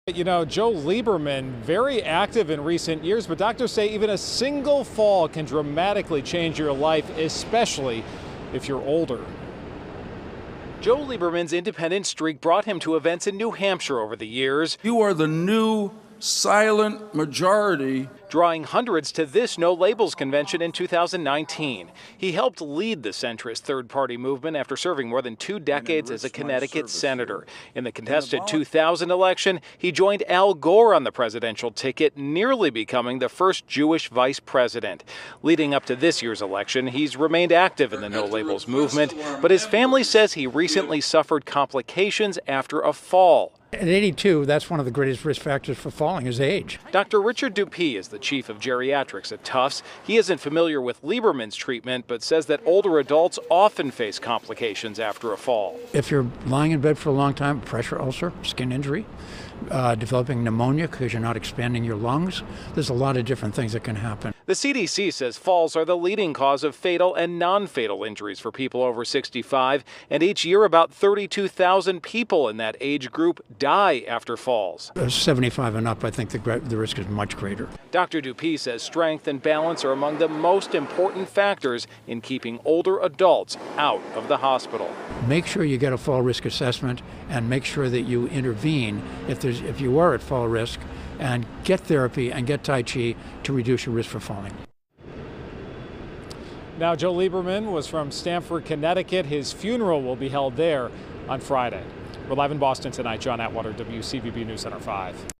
Lieberman, longtime senator from Conn., dies of complications from fall
The CDC says falls are the leading cause of fatal and non-fatal injuries in adults 65 and older. Former Sen. Joe Lieberman turned 82 last month.